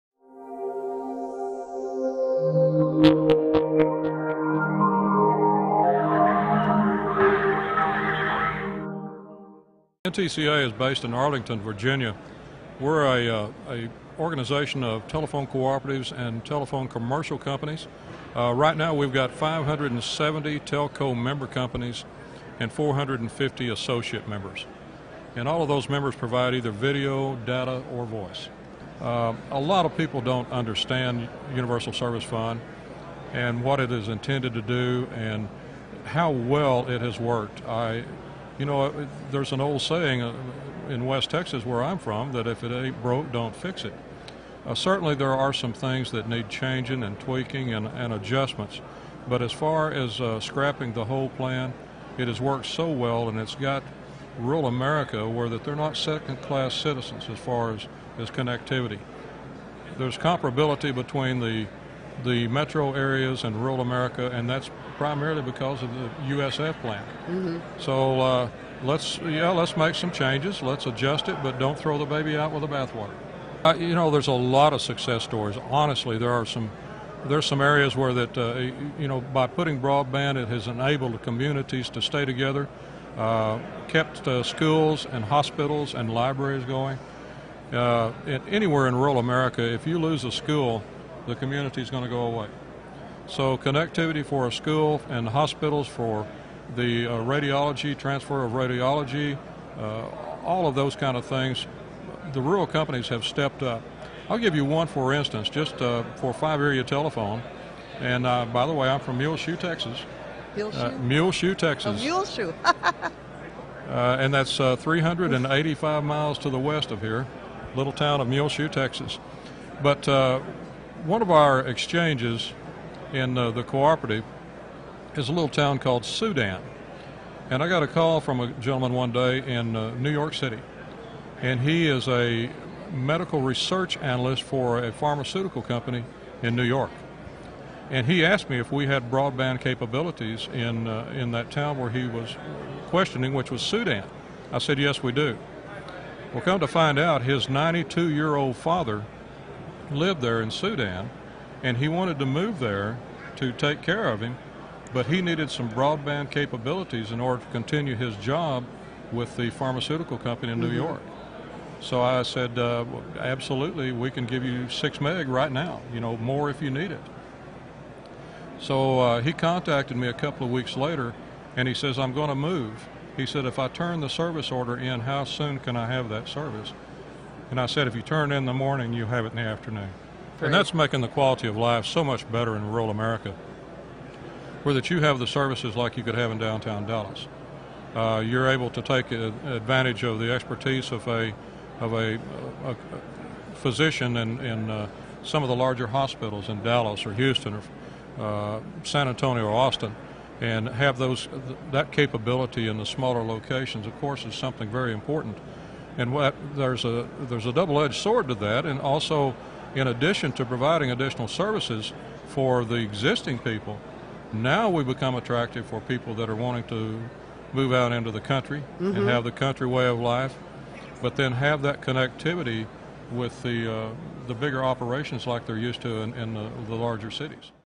TIA 2011: Sandy Vandevender, President, NTCA
Sandy Vandevender, President, NTCA talks on the Universal Service Fund and providing broadband service to rural areas.